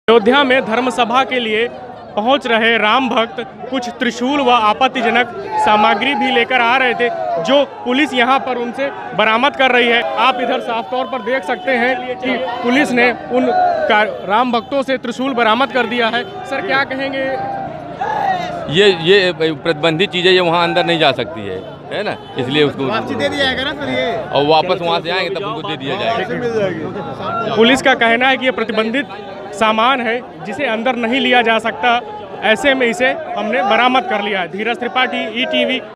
अयोध्या पहुंच रहे राम भक्तों के पास से पुलिस ने बरामद किए त्रिशूल
अयोध्या : राम मंदिर निर्माण के मुद्दे पर विश्व हिंदू परिषद आज सुबह 11 बजे से धर्मसभा का आयोजन कर रही है. आयोजकों का दावा है कि यहां करीब 3 लाख राम भक्त जुटेंगे. देशभर से लोगों की भीड़ पहुंच रही है और सुरक्षा के कड़े इंतजाम किए जा रहे हैं. चेकिंग के दौरान पुलिसकर्मियों ने भक्तों के पास से त्रिशूल बरामद किए हैं.